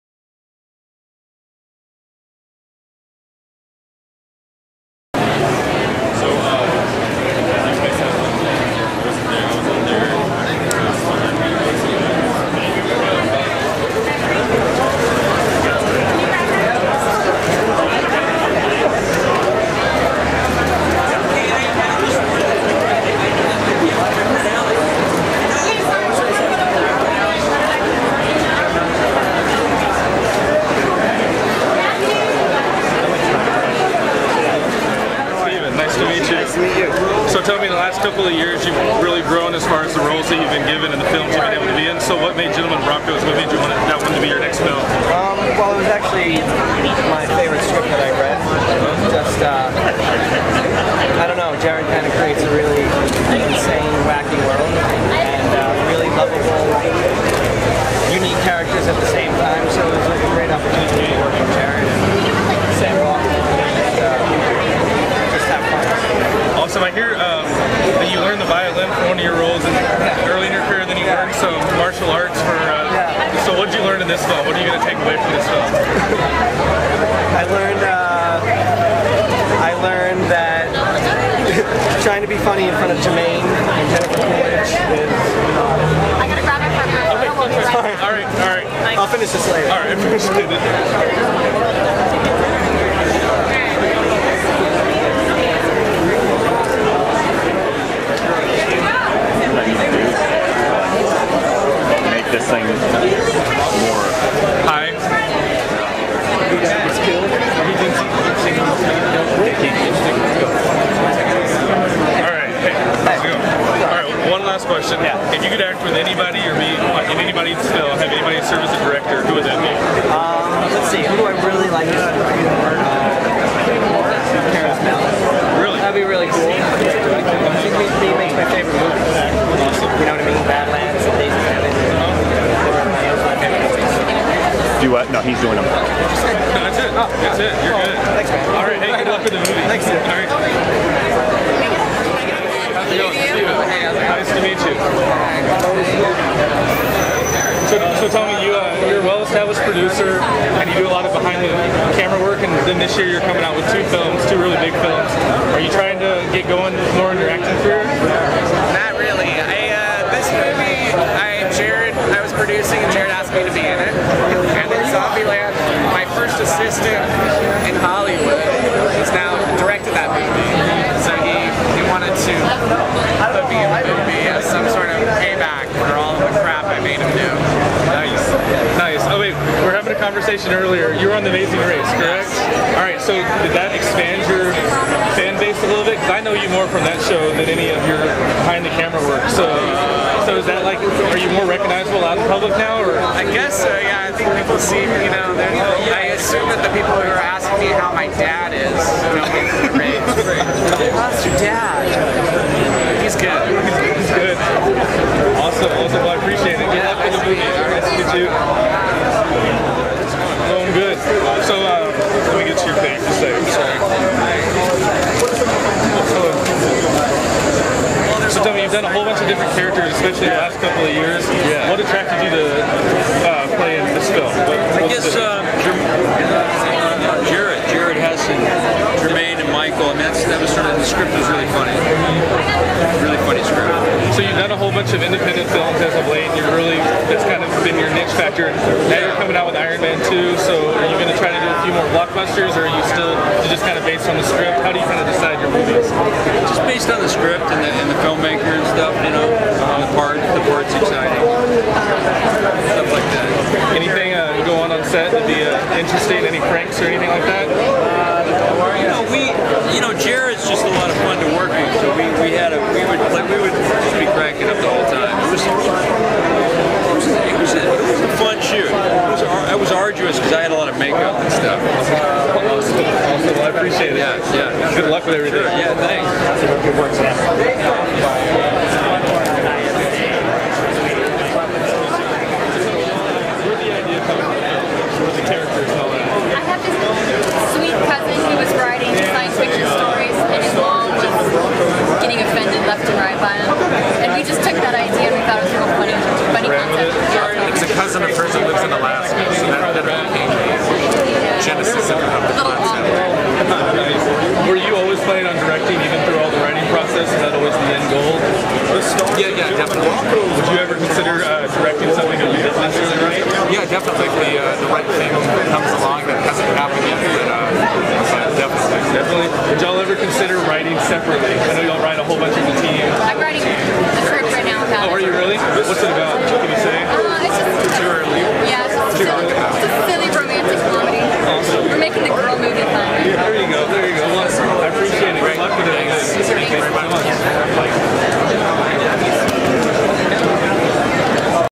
GENTLEMEN BRONCOS World Premiere Austin, TX
Fox Searchlights GENTLEMEN BRONCOS World Premiere in Austin, TX for Fantastic Fest. Interviews include Director Jared Hess and Jerusha Hess, Mike White, Michael Angarano and Sam Rockwell. Austin, TX Sept 24, 2009